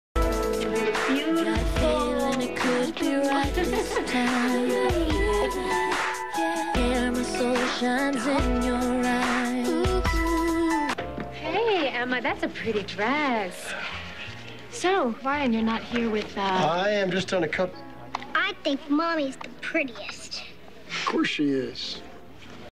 9-24-08.
Kathy goes to Babe and JRs engagement party